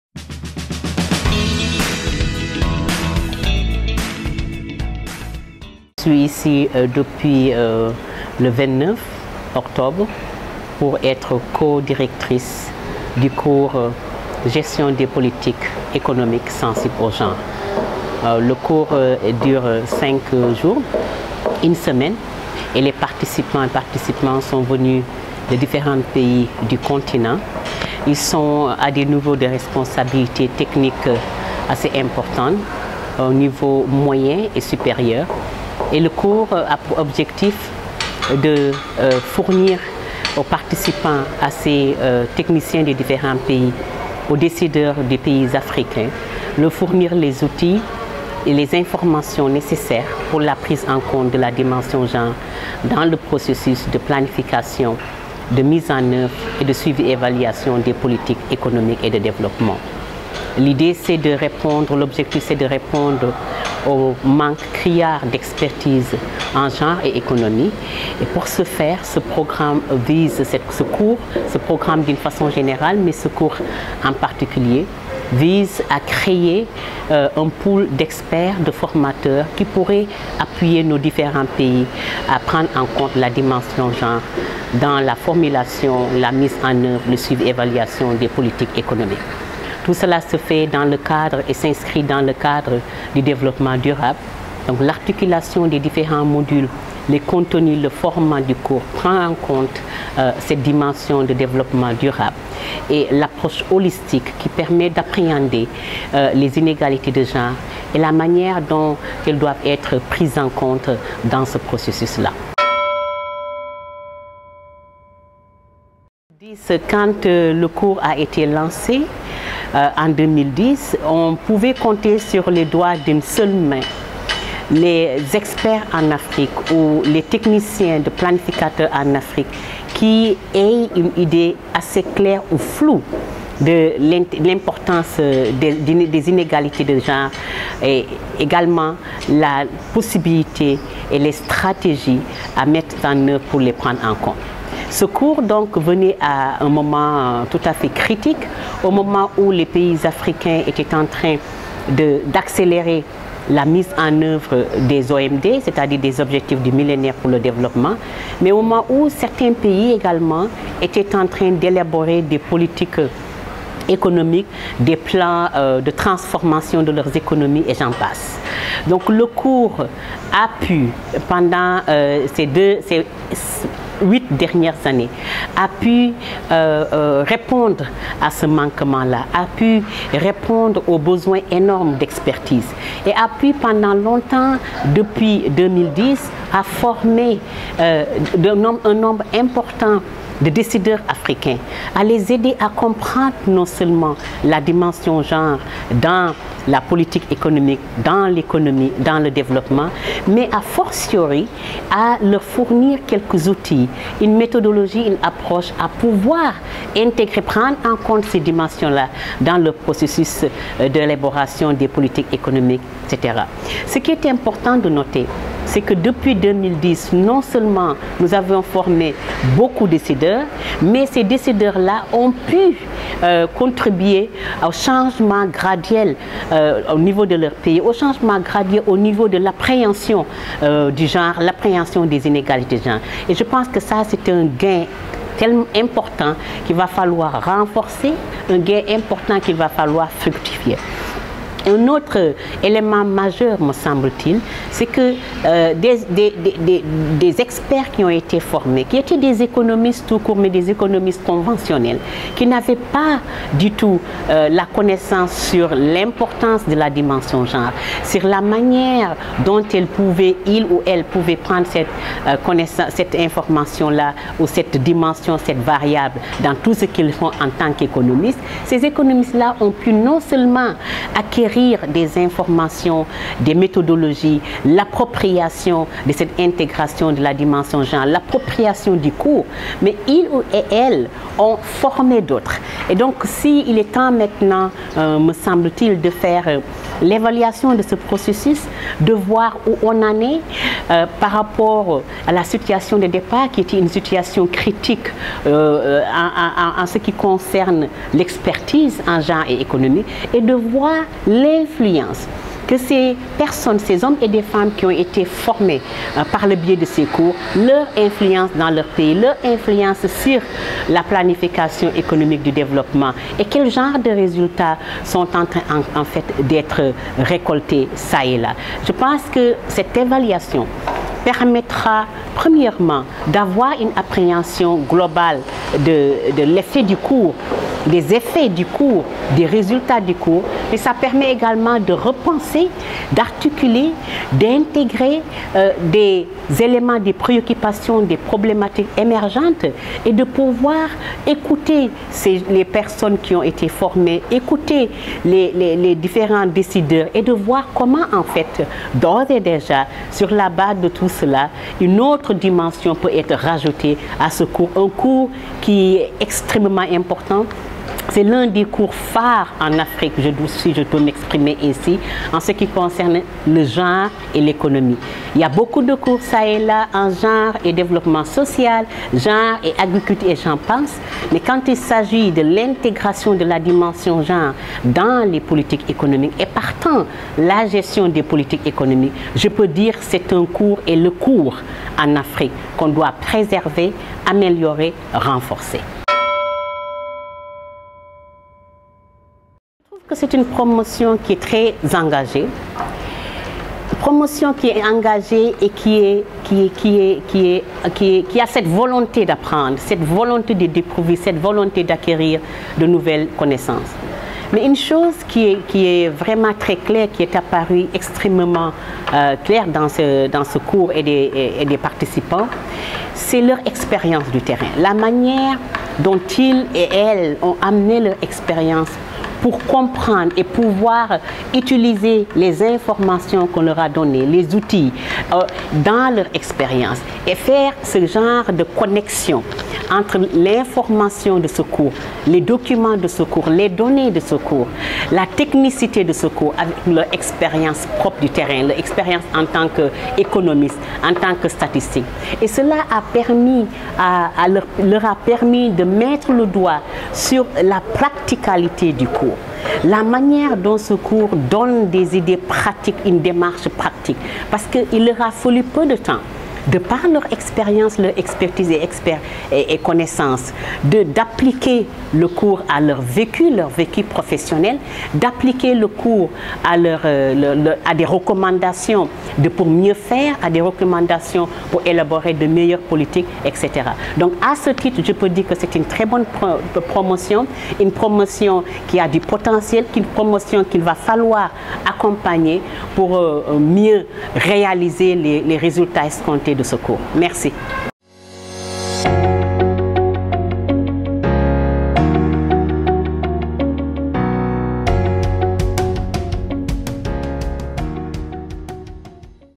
Mme Ngoné DIOP, Directrice de cours, Spécialiste de la question Genre ECA
Atelier de formation de formateurs sur la gestion de la politique économique sensible au genre - Dakar, 29 Octobre - 02 Novembre 2018.